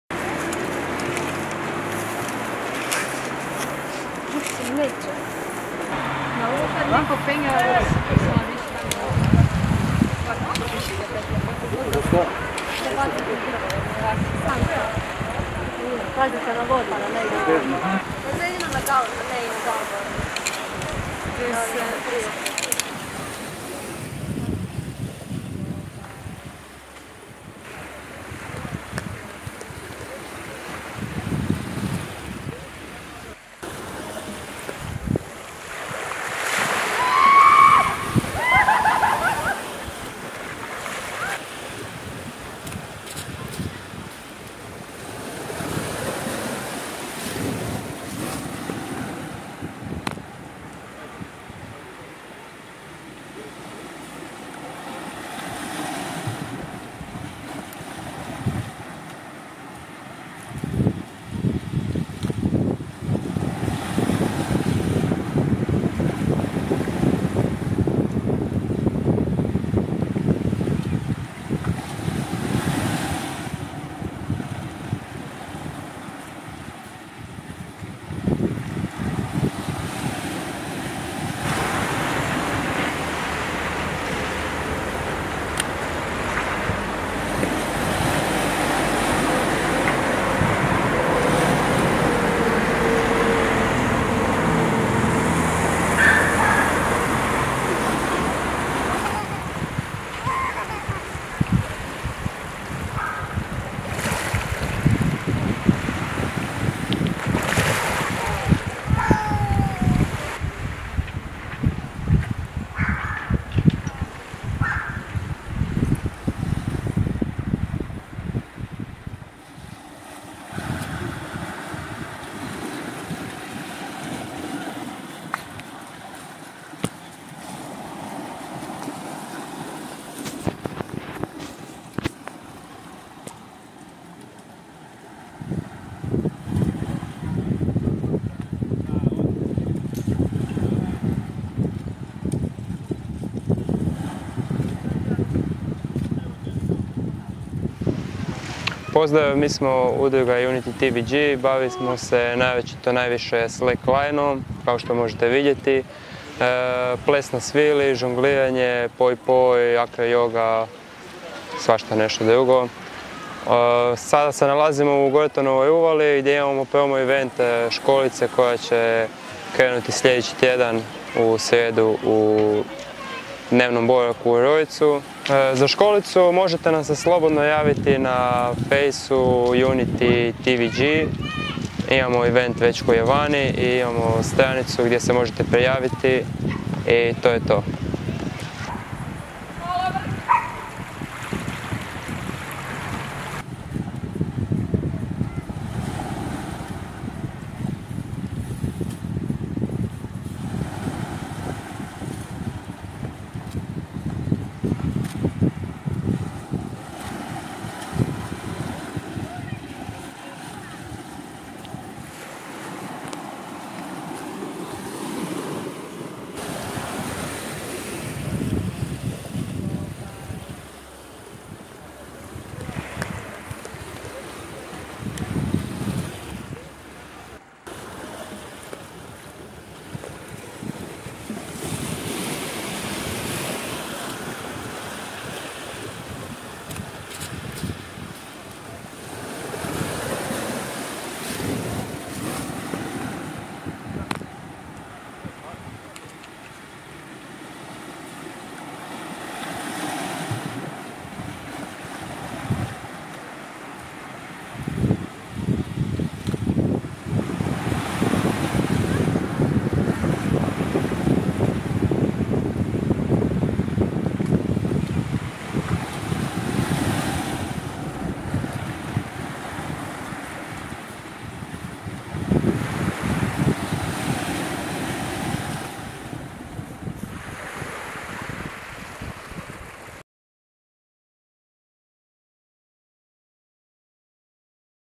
Vježba highlininga u Gortanovoj uvali | PIXSELL PLUS
Udruga Unity TWG danas je u Gortanovoj uvali na gurtni koja je razapeta po uvali iznad mora pokazivala svoje vještine highlininga i tako se reklamirali za malu školu koji će se održati u rujnu i listopadu. Na tečaju će se polaznici upoznati sa osnovama Slacklininga i Highlininga.